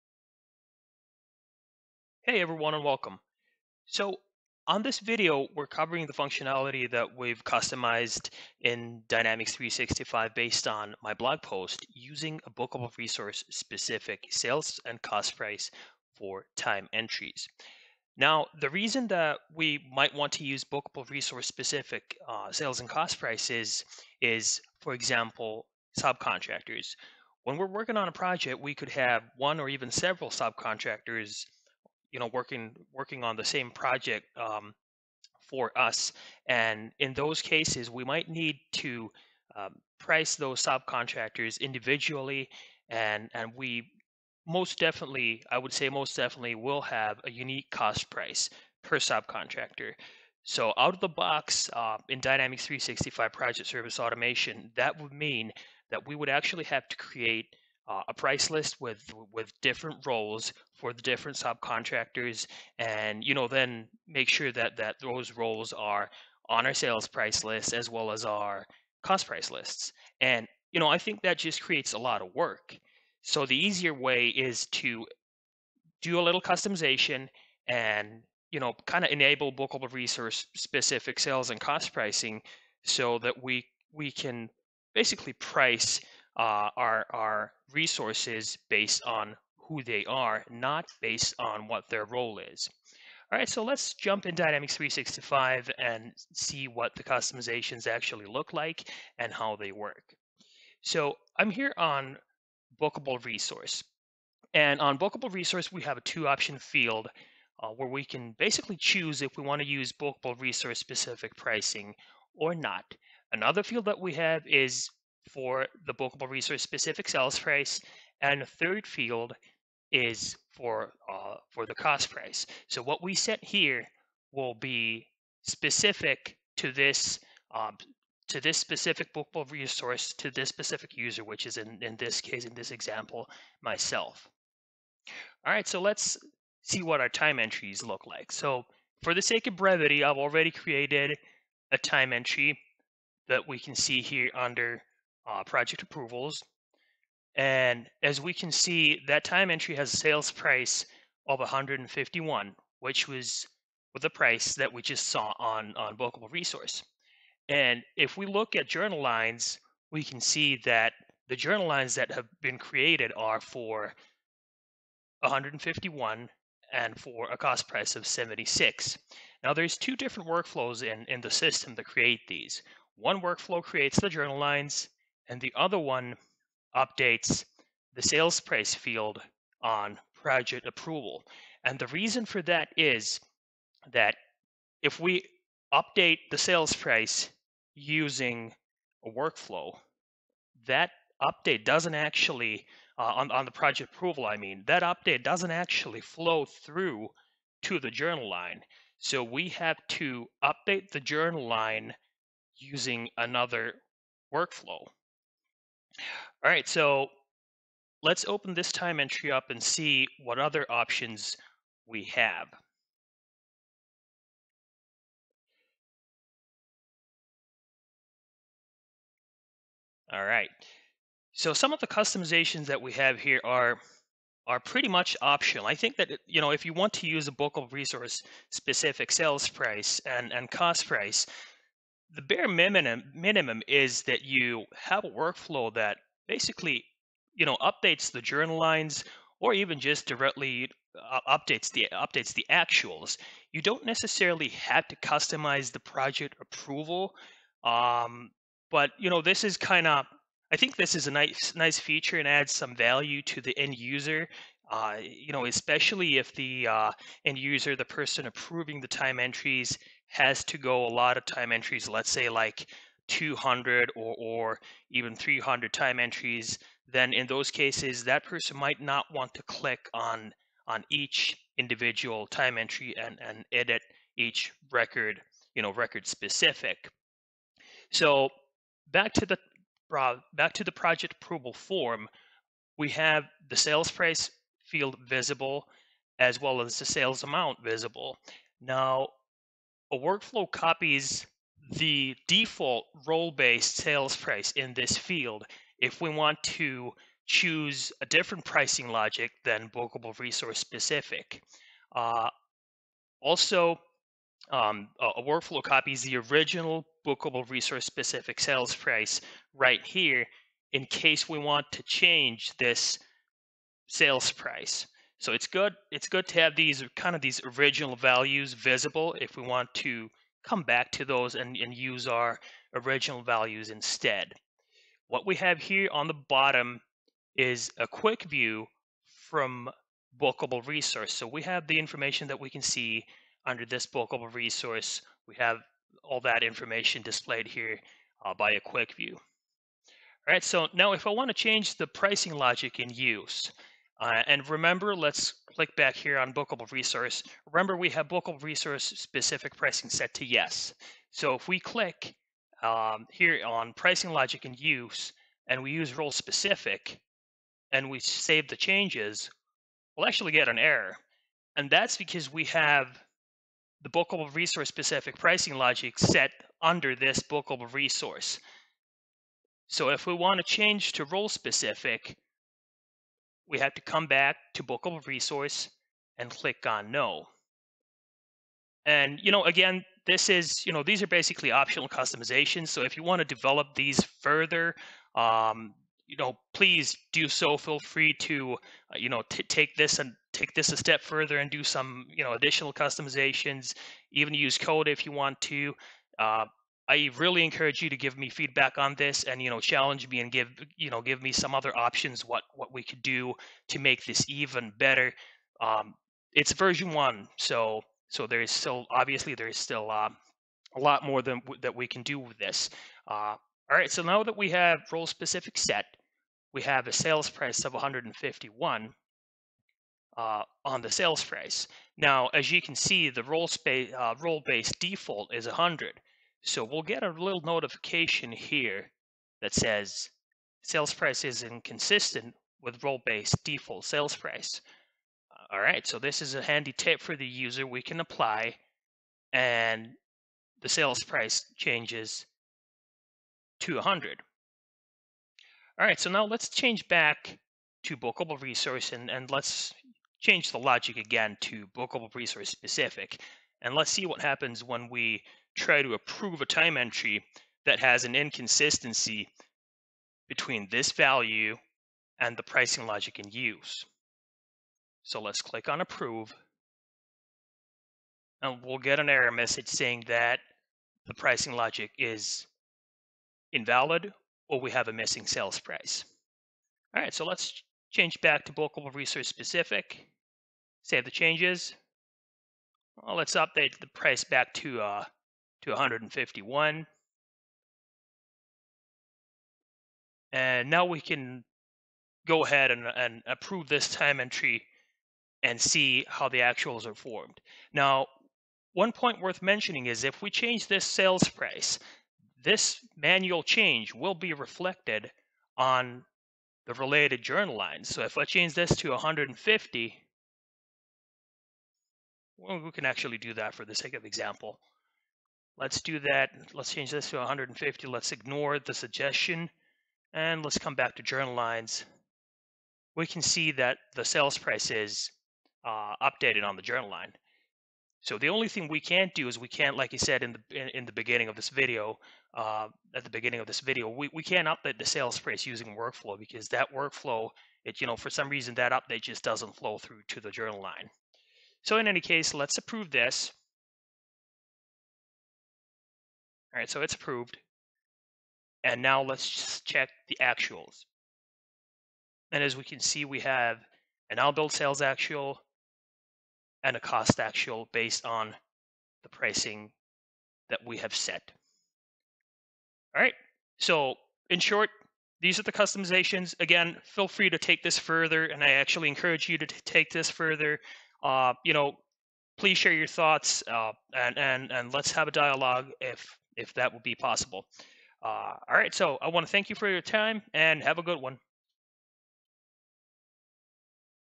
D365 PSA - Bookable Resource specific Sales and Cost Prices
This video walks your through the functionality of using Bookable Resource specific Sales and Cost Prices in Dynamics 365 Project Service Automation. Follow the steps in my blog post to customize your Dynamics 365 to enable this functionality: